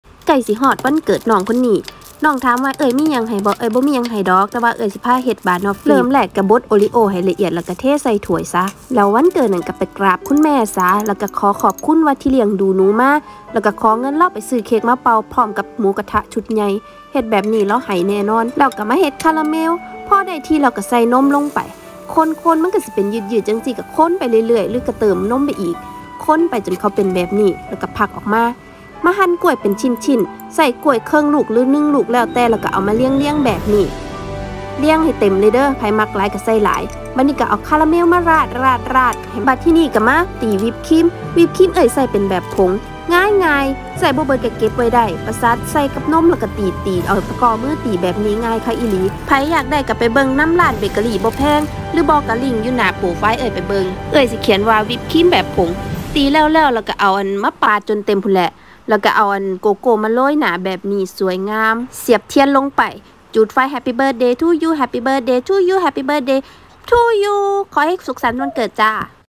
บานอพฟี่วันเกิดแบบง่ายๆ
มีน้องในติ๊กต๊อกใกล้ถึงวันเกิด